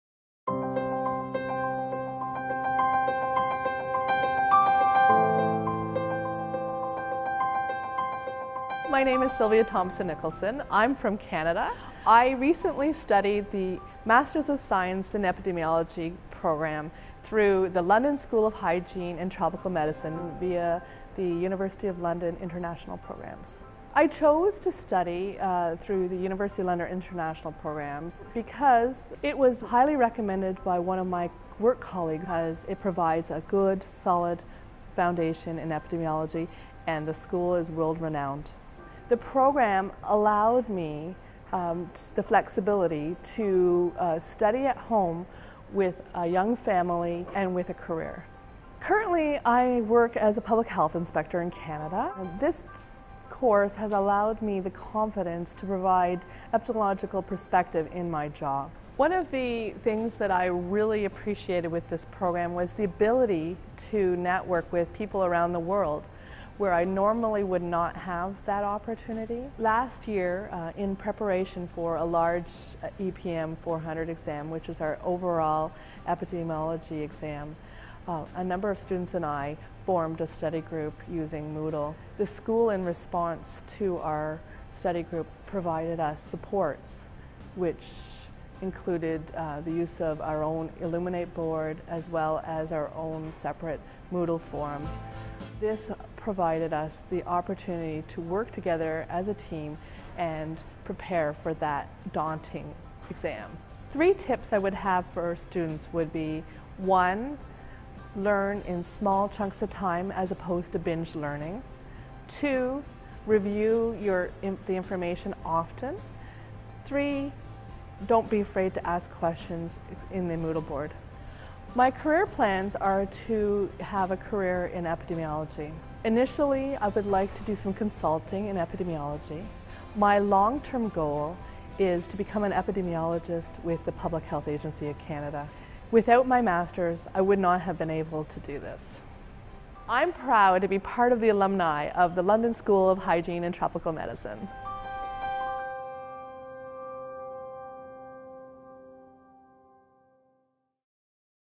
Alumni Inspiration - Silvia Thompson-Nicholson - MSc Epidemiology
Silvia Thompson-Nicholson talks about her experience studying MSc Epidemiology in Canada, through our distance learning program.